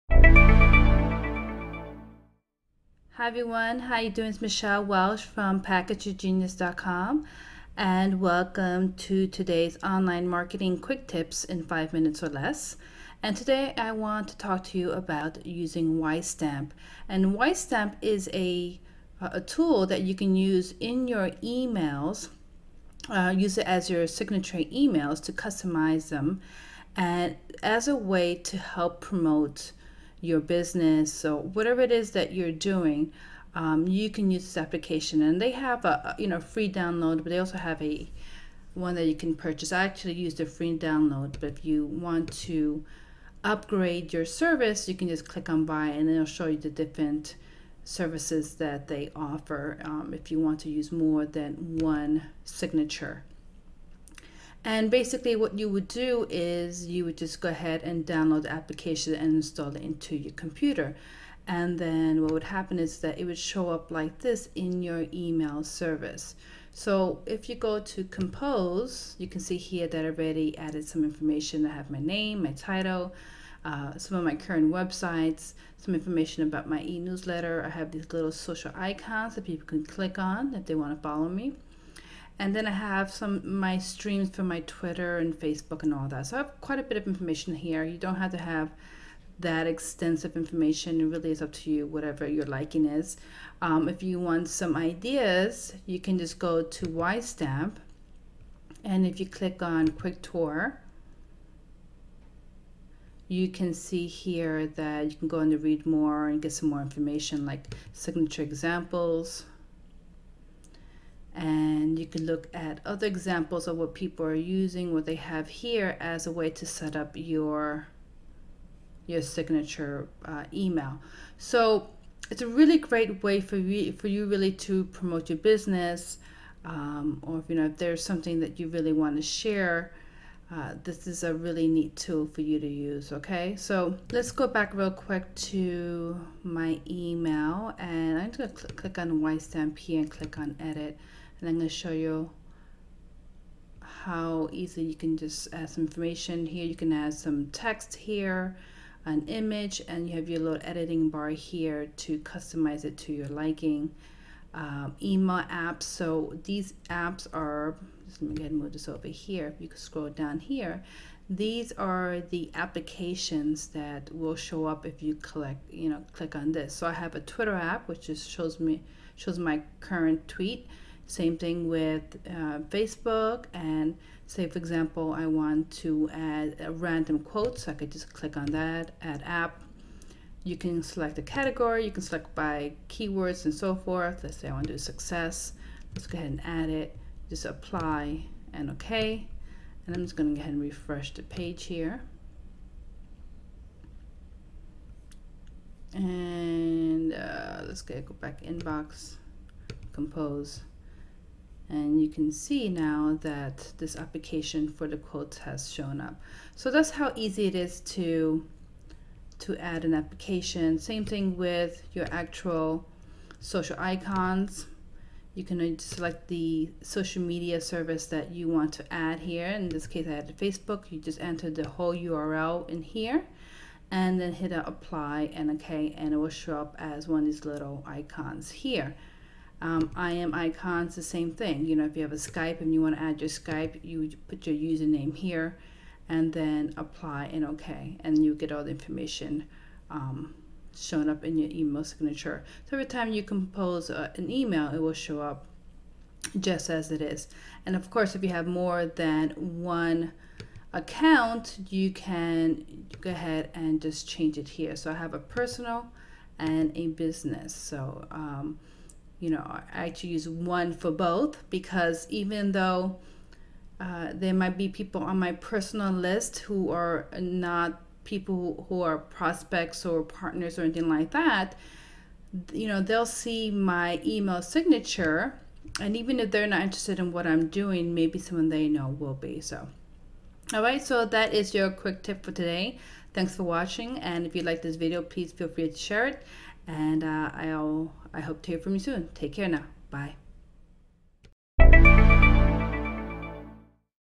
Using Wisestamp for Your Email Signature
In today's Online Marketing Quick Tip in 5 Minutes or Less, I discuss how you can use Wisestamp to create a dynamic and social-rich signature.

This tactic has many benefits:

    -It allows you to engage those that read your email
    -Brings awareness to your message/business
    -Creates the potential for referrals
    -Amongst others...

In this video on 'How to use Wisestamp to maximize your email signature', I cover:

    -How to download the application
    -How to edit Wisestamp to include your social icons
    -How to add customize text and images
    -How to add third-party applications